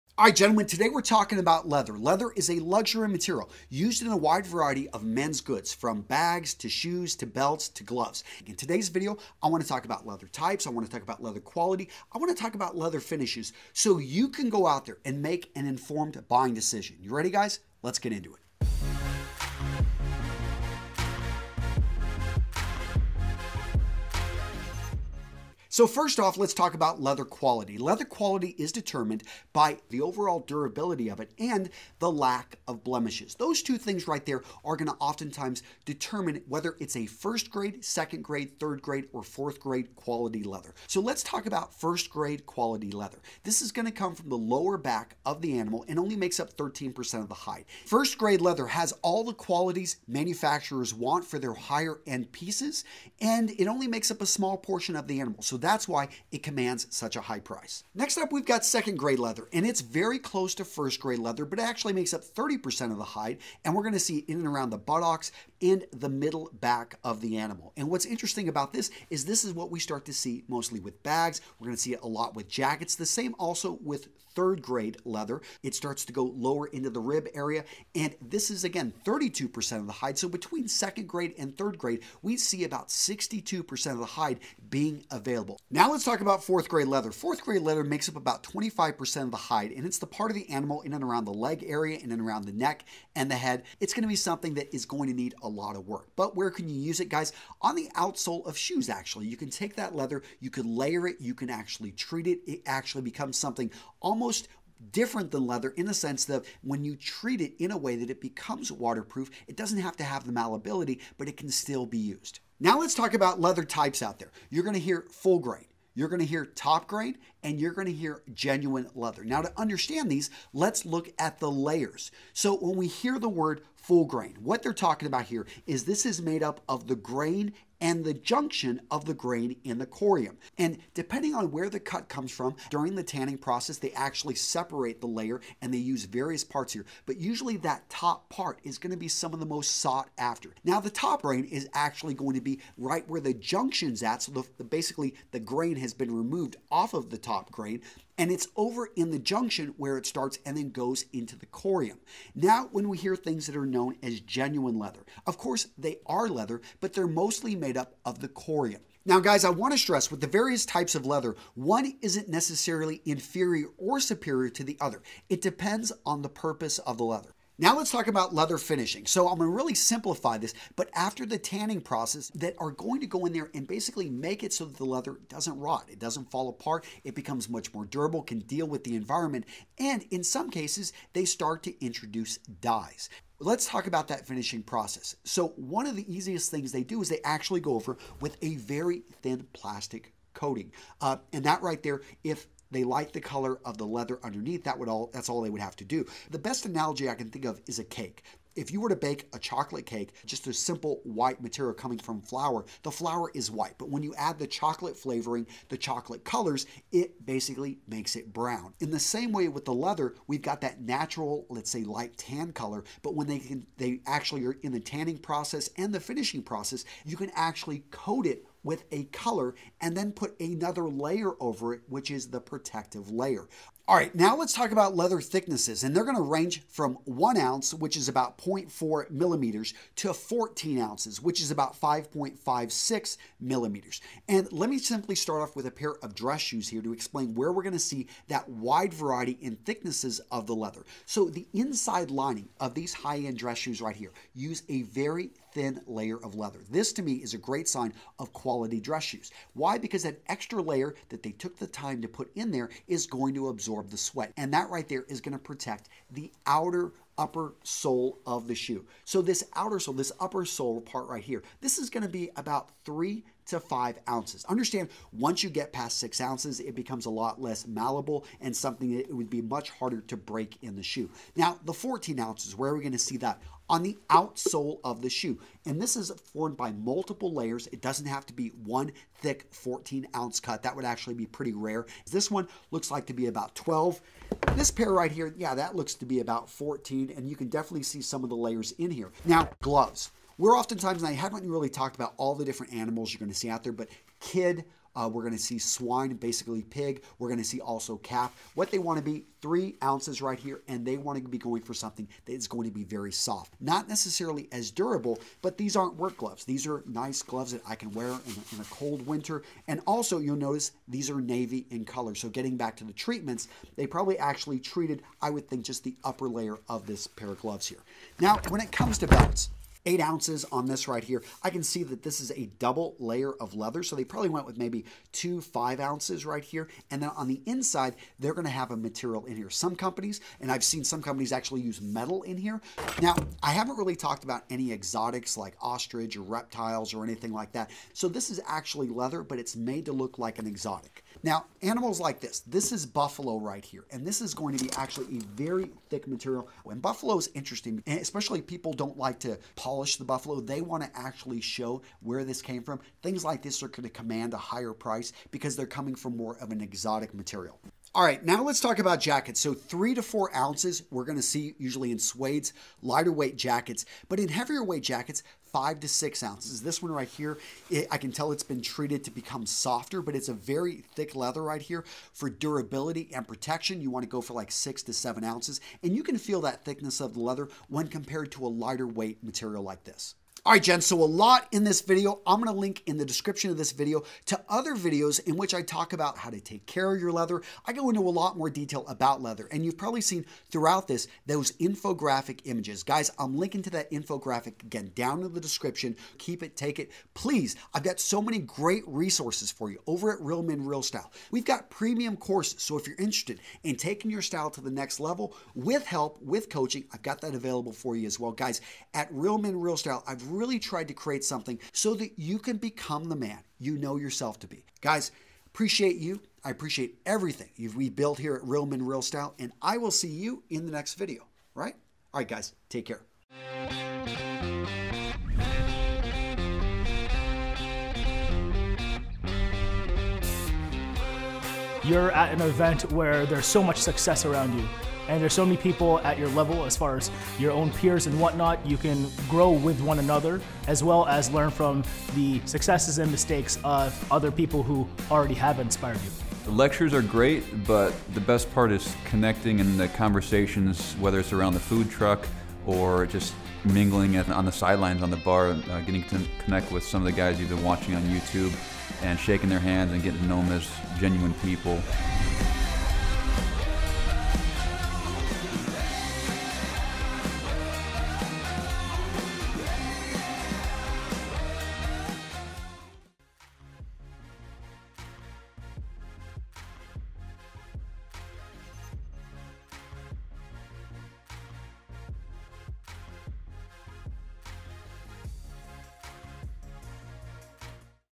ULTIMATE Guide To Leather! Full Grain Vs Top Grain Vs Genuine Types & Grades
Ever spent a fortune on new leather shoes, only to watch them start peeling like a sunburned cow after just five minutes?. What did you waste your money on? And what can you to make sure it NEVER happens again? In today's video, we're going to break down the different types of leather, what they're used for, and what you should be looking for when you're buying leather shoes, gloves, jackets and bags.